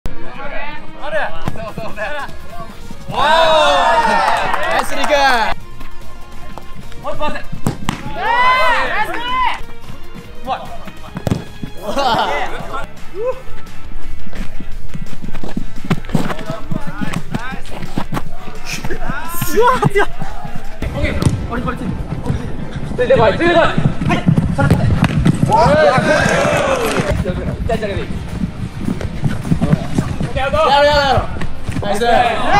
【WINNER'S】FC LISEMのセレクションに参加する森保陸のプレー集！！！【FC LISEM】【森保陸】
【FC LISEM#3】セレクション開幕！1vs1•2vs2で己の強さ•デュエル能力を魅せろ！リゼムの予想を上回る超ハイレベルなサバイバルゲームスタート。